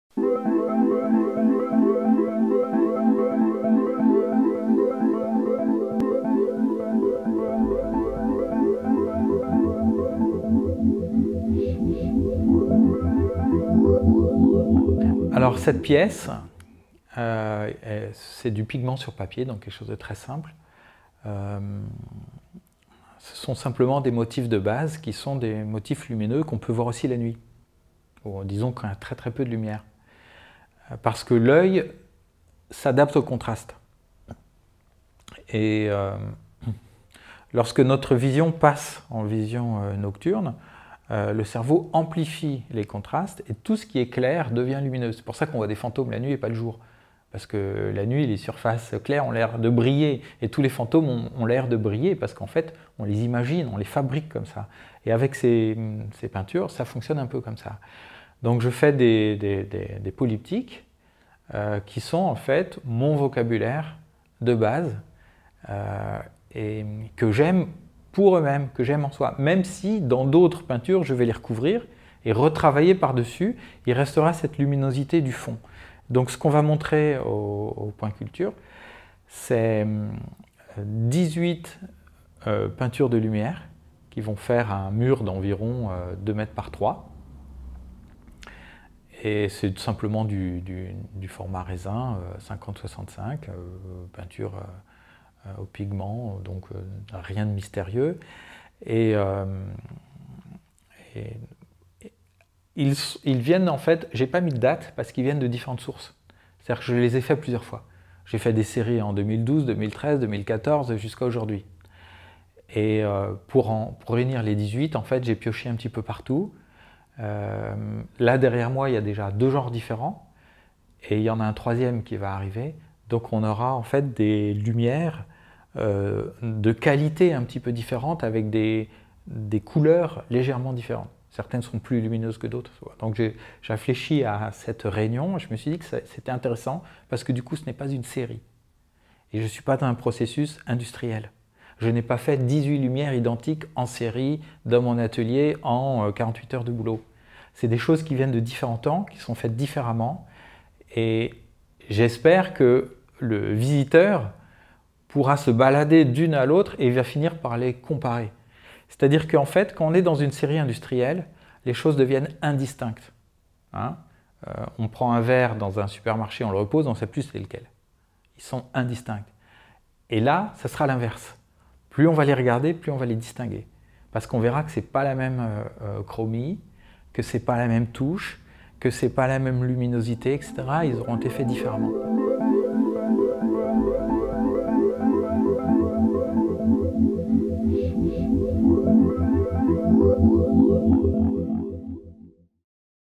7M² | Hervé Ic
De la lumière en peinture pour inaugurer la quatrième saison du 7m²
Hervé Ic a réussi un défi de taille : produire une peinture lumineuse sans éclairage et induire de la profondeur sans aucune perspective. L’artiste crée des espaces picturaux où chacun peut évoluer librement grâce aux effets de transparences. Hervé Ic brouille les pistes avec ses points lumineux qui se confondraient presque avec des spots qui rayonnent sur scène.

Le dompteur de lumière nous explique sa conception de l’art et sa méthode de travail :

« La plasticité que l’on travaille n’est pas celle de la peinture, c’est celle du cerveau qui est un organe plastique et qui s’invente des jeux. Quand je commence un tableau, je connais mon sujet, mais je ne connais pas l’image qui sera créée. Cette image est le résultat d’une partie à chaque fois différente. L’improvisation en peinture, coule de la vie à l’œuvre, sans que l’on puisse véritablement distinguer leur limite. Elle est fondamentale. Si une œuvre devient une production calculée et mécanisée - c’est-à-dire industrielle - l’art n’existe plus ». — Hervé Ic

À PROPOS D'HERVÉ IC :

Hervé Ic est un peintre français vivant et travaillant à Bruxelles. Il a étudié les technologies de l’image ainsi que l’intelligence artificielle appliquée à l’'image (DEA IARFA) aux universités Paris VIII et Paris VI. Il se consacre aujourd’hui pleinement à la peinture.